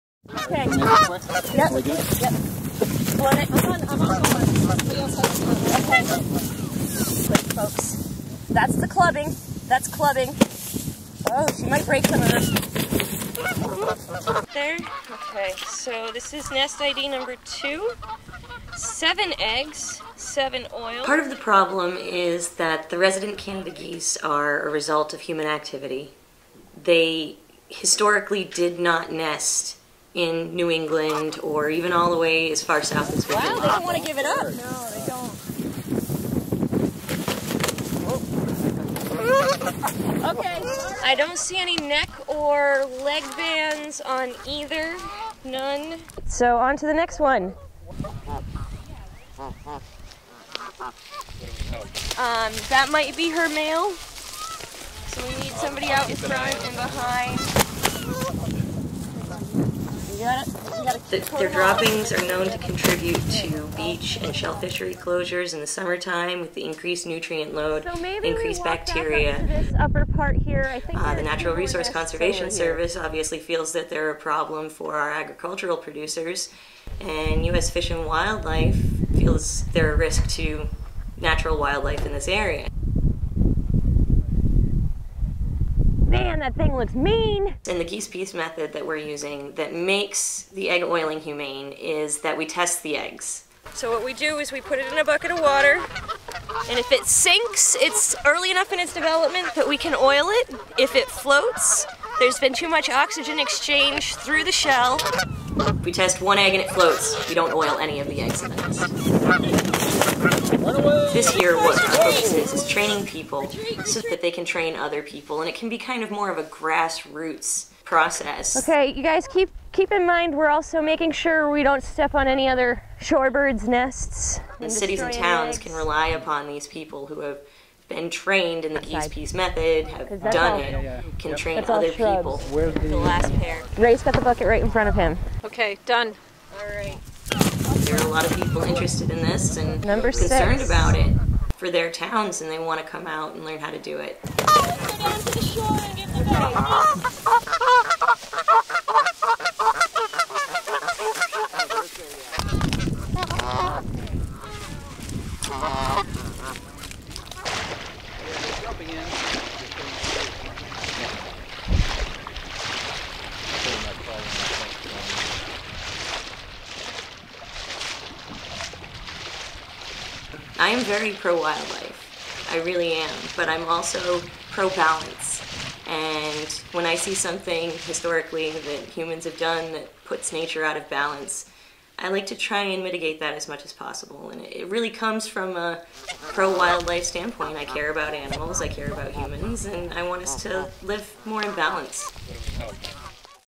'GeesePeace': Taking RI's Canada goose population in hand
Jessica Blackledge, of the Eastern Rhode Island Conservation District, leads a hands-on training session for volunteers on how to locate Canada goose nesting sites and perform egg oiling to manage to the goose population. Volunteers locate 7 nests on Walker Island in Bristol and manage to oil the eggs in six of them.  The process is approved by the Humane Society and by PETA. Providence Journal video by Sandor Bodo